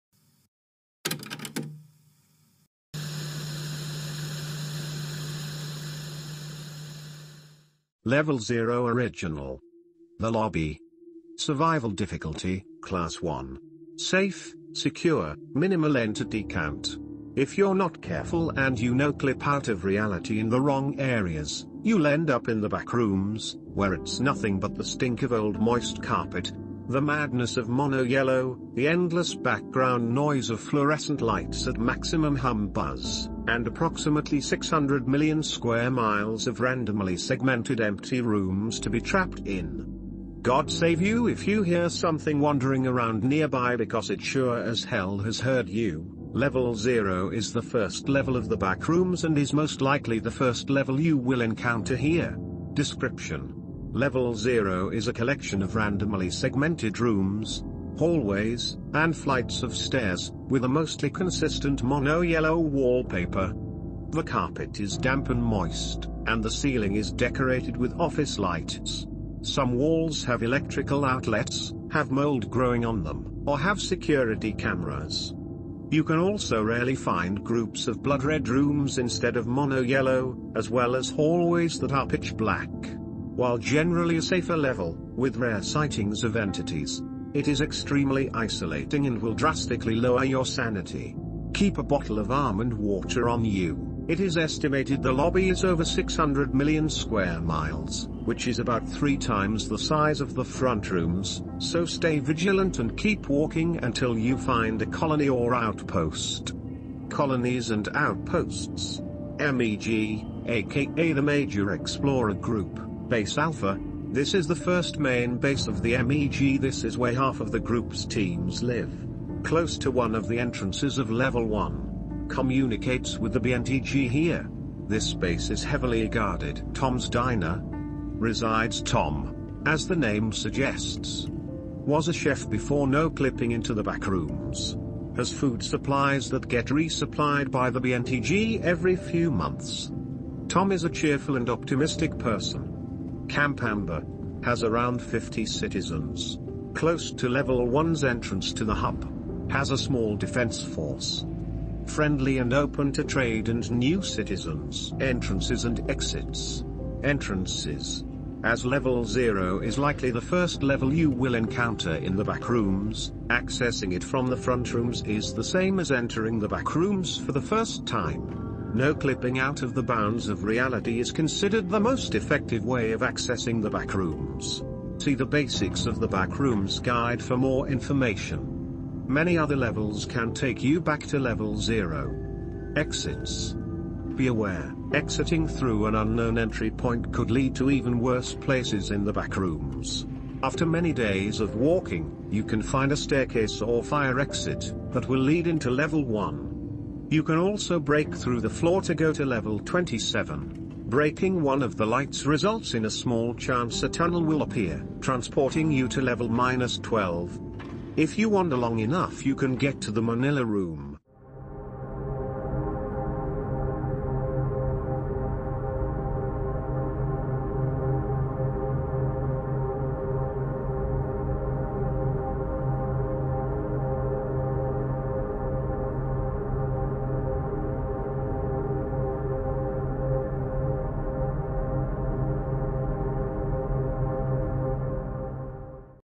Level 0 "The Lobby" (original version) | Levels of The Backrooms
I will be uploading Backrooms, voidclusters and SCP readings typically daily

This entry was adapted by Reddit user u/Deveyerr, and rewritten by 1000dumplings